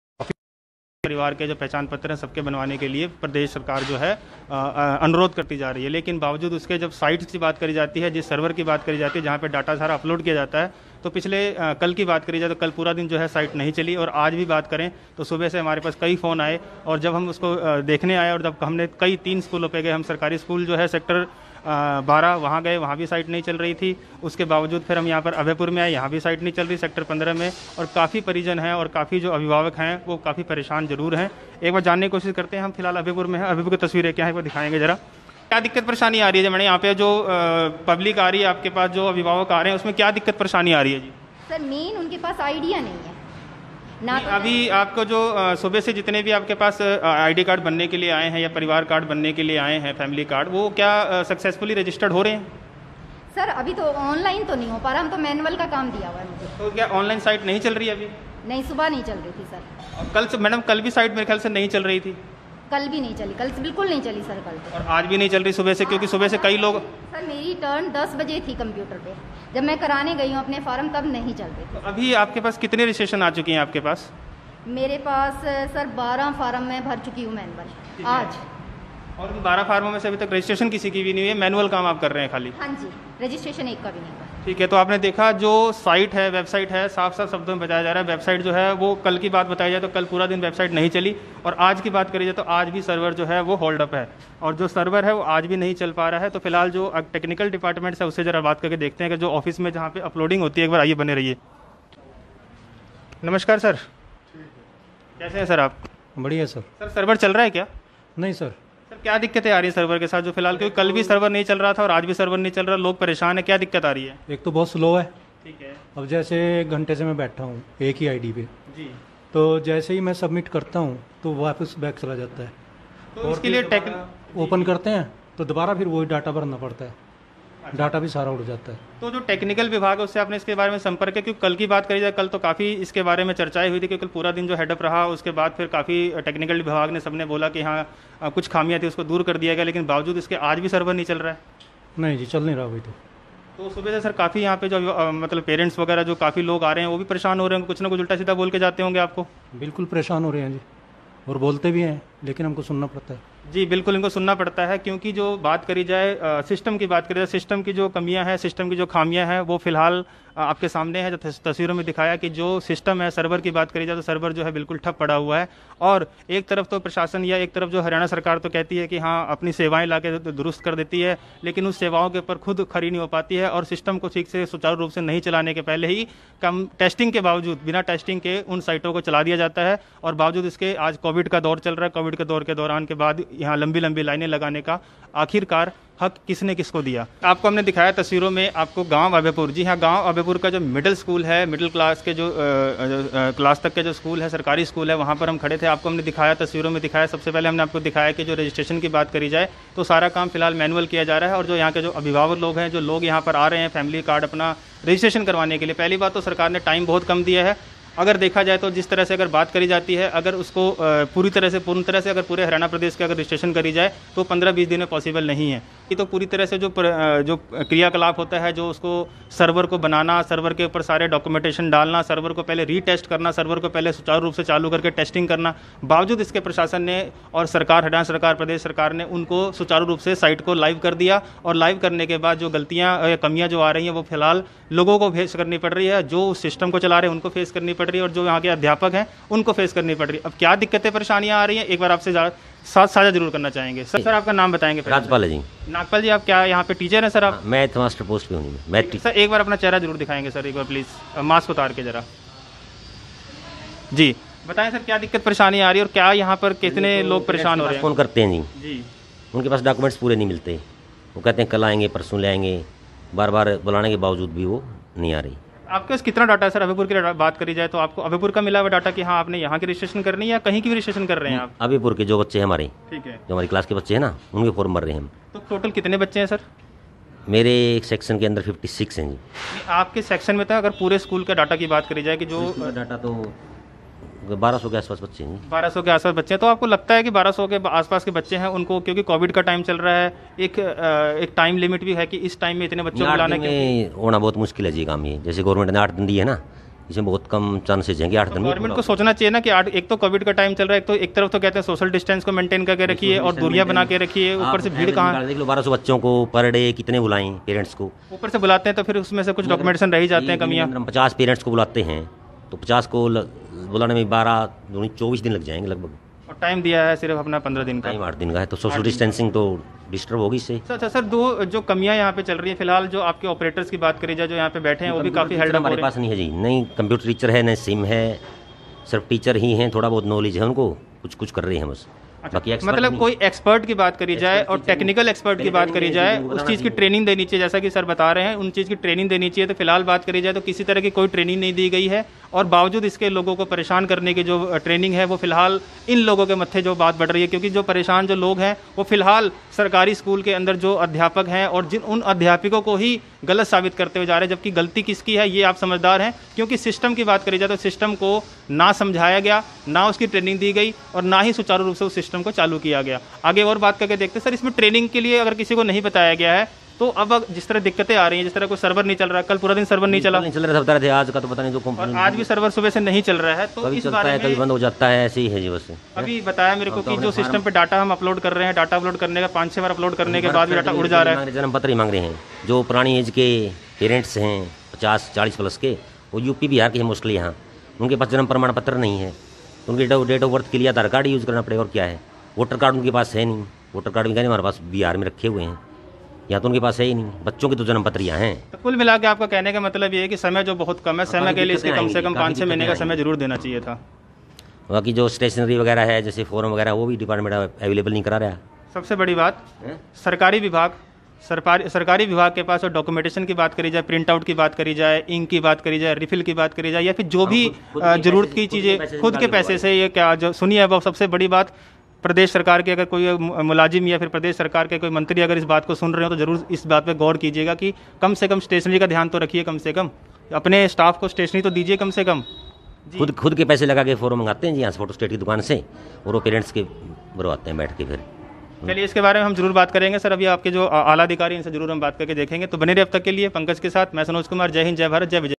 Panchkula News Live : परिवार पहचान ID बनवाना और बनाना अभिभावकों व अध्यापकों के लिए बना सिर का दर्द
Panchkula Update , Live Update
हरियाणा परिवार पहचान पत्र बनवाना है तो ये खबर आपके लिए है , एकबार देखिये जरूर और शेयर करना न भूलियेगा ।